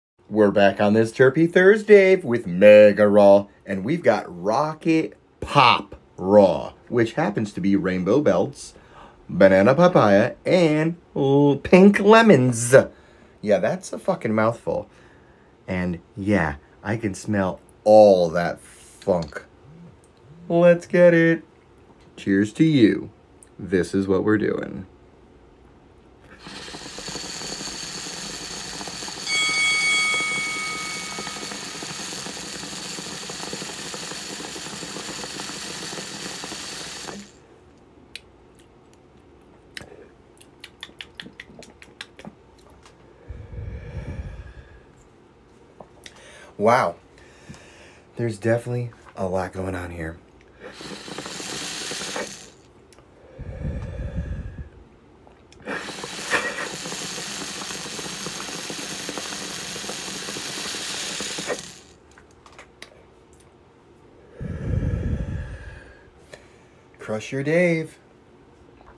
Happy Terpy ThursDave, Mega Raw Melts - Rocket Pop Raw, NOTHING FOR SALE OR TRADE… 8.3 🔥
Happy Terpy ThursDave, Mega Raw Melts - Rocket Pop Raw, NOTHING FOR SALE OR TRADE… 8.3‼️ Rainbow Belts ❌ Banana PiePaya ❌ Pink Lemonz is the genetic cross… Wow, lots of flavors in one 1g jar, and a desirable consistency. So I opened this jar and as expected it smells great… For my temps for dabbing I went in at 525/515/505 and was able to enjoy the sweet & fruity candy flavors, w hints of earthy and 🍋… Overall for an exhale I was getting tropical fruity, lite grapefruit sweet taste… First my mind was instantly high then the body over, overtook the nice buzz effect… I read this strain eases the mind, and can add in it gave my body full relaxation… This strain aids w mostly stress, anxiety, and depression… Sending good vibes and ☁️💨 to everybody til next time…

Ps. Mega Raw Melts really came in 🥵 w the 🔥 rosin…